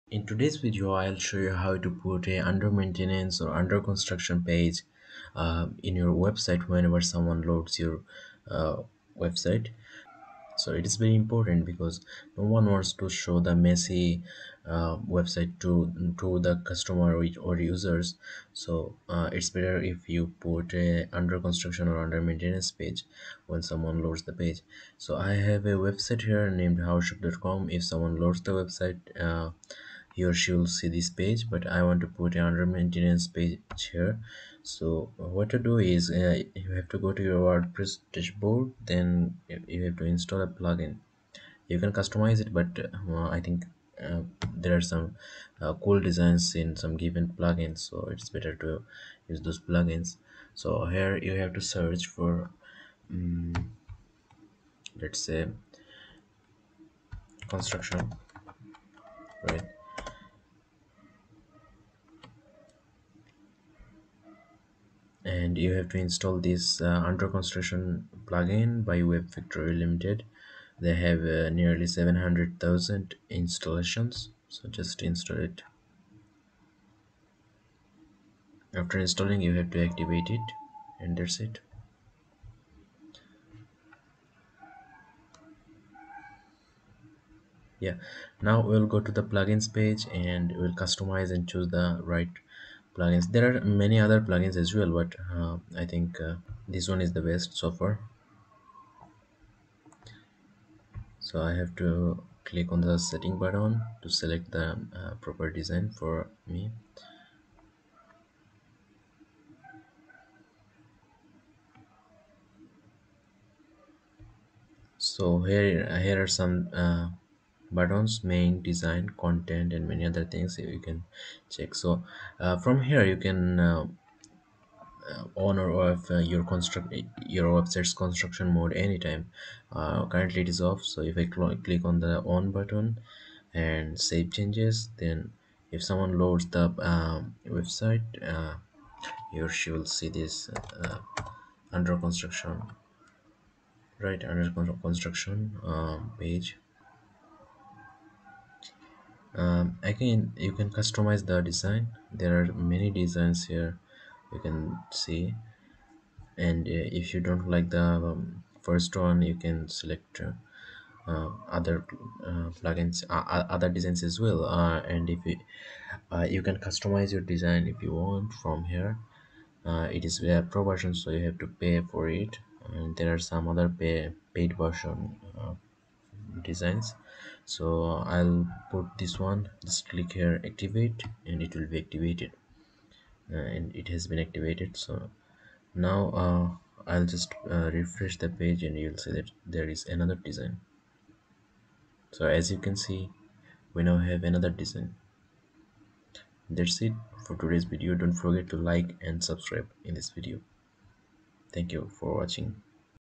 🚧 How To Add Under Construction / Maintenance Mode in a WordPress Website || Shafi Academy
Learn how to seamlessly implement under construction or maintenance mode on your WordPress site with this easy tutorial. Keep your site looking professional while you make updates behind the scenes. 🛠️👩‍💻